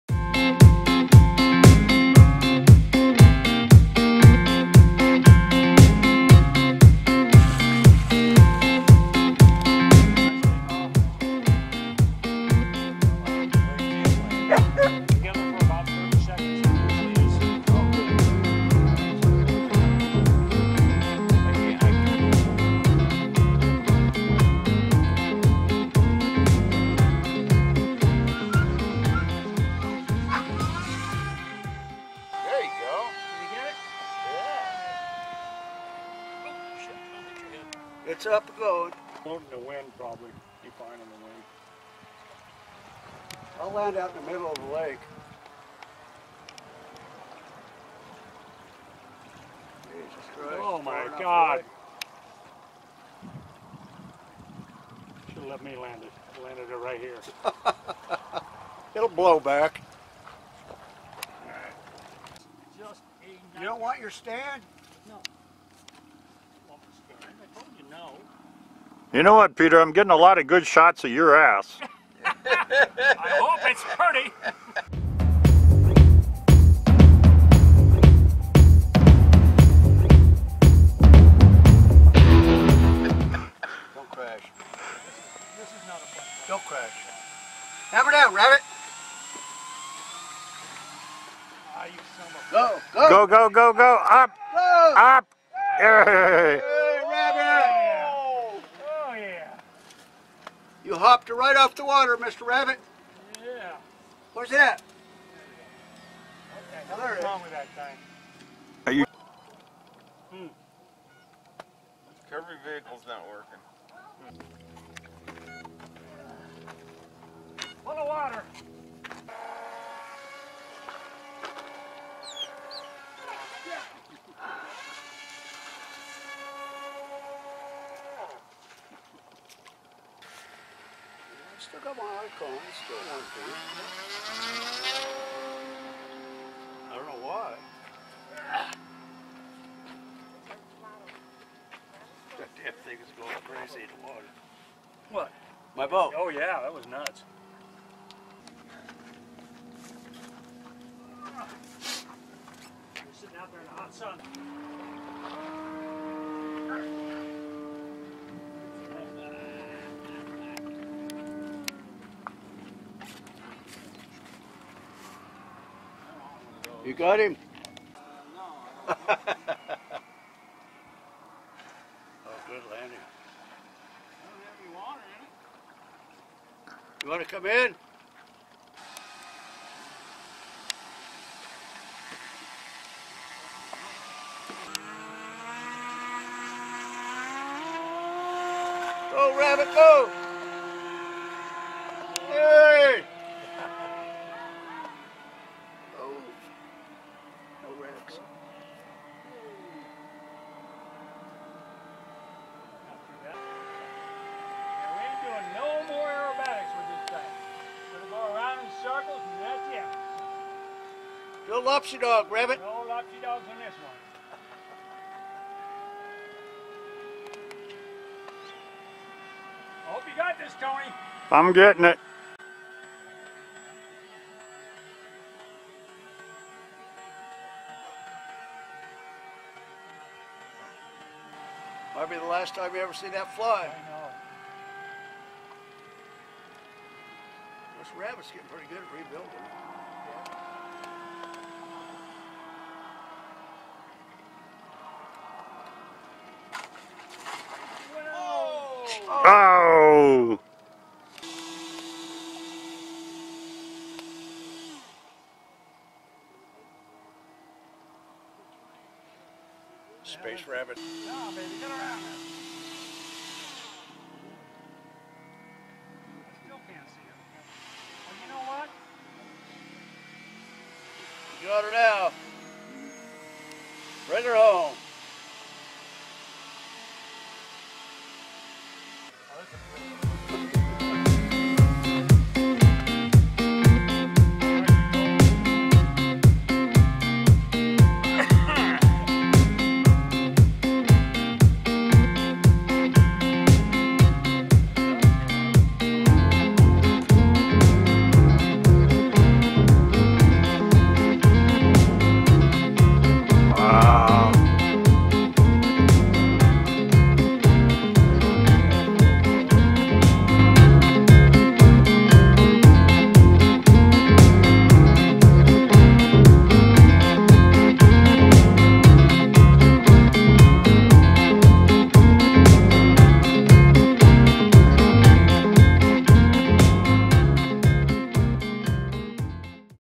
2018-04-26 Float Fly
Fourth of our ongoing Overton Float Fly's.  Each one gets better and better.